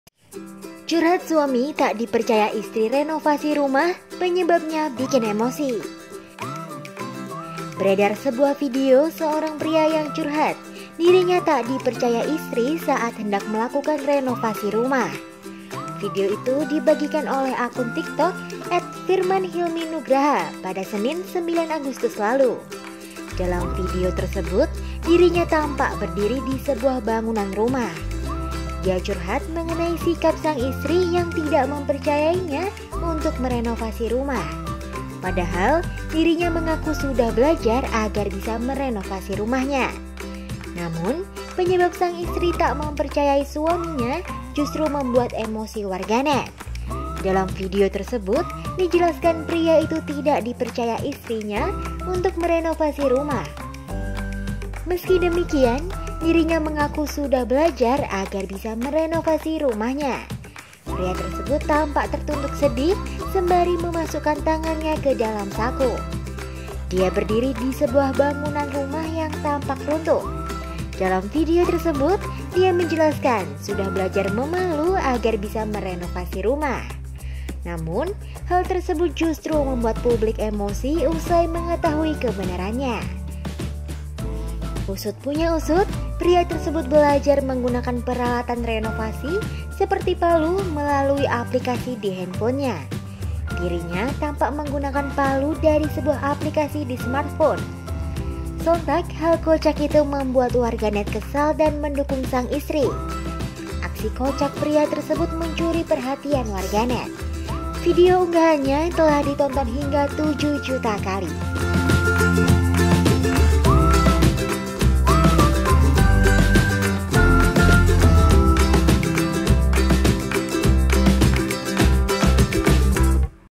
Curhat Suami Tak Dipercaya Istri Renovasi Rumah, Penyebabnya Bikin Emosi
Beredar video seorang pria yang curhat dirinya tak dipercaya istri saat hendak melakukan renovasi rumah.

Video itu dibagikan oleh akun TikTok @firmanhilminugraha, Senin (9/8/2021). Dalam video tersebut, dirinya tampak berdiri di sebuah bangunan rumah.

Dia curhat mengenai sikap sang istri yang tidak mempercayainya untuk merenovasi rumah. Padahal dirinya mengaku sudah belajar agar bisa merenovasi rumahnya. Selengkapnya dalam video ini.

VO/Video Editor: Lintang Larissya/ Yulita Futty Hapsari